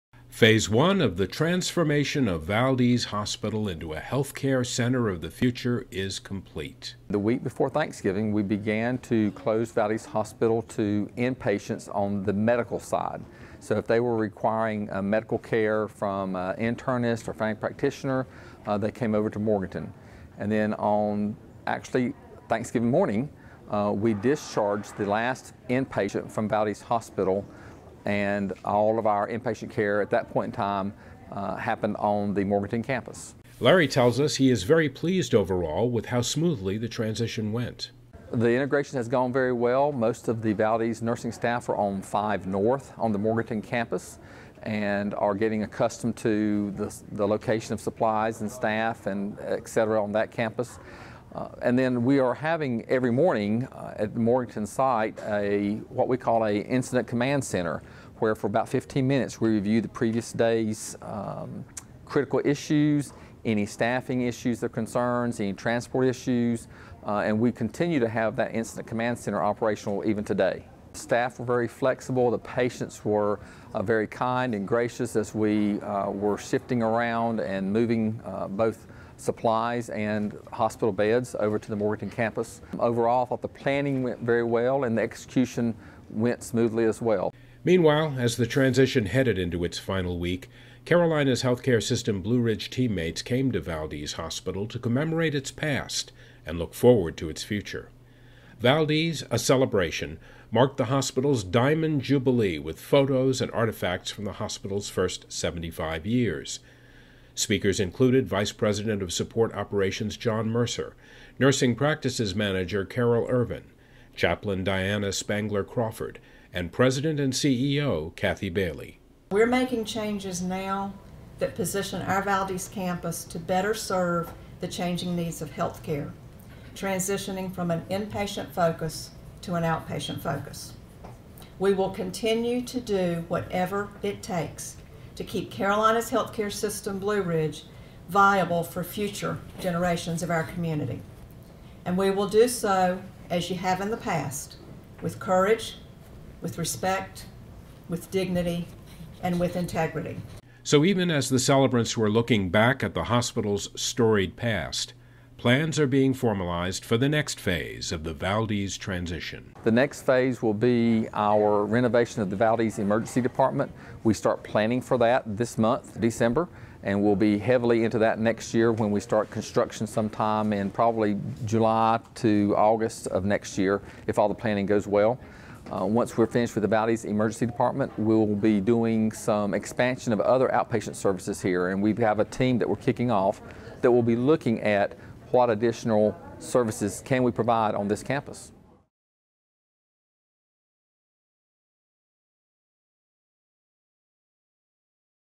Valdese Hospital - Transition & Celebration
The first phase of the transformation of Valdese into a "Healthcare Center Of The Future" successfully concluded one week after the celebration of the hospital's 75th Anniversary.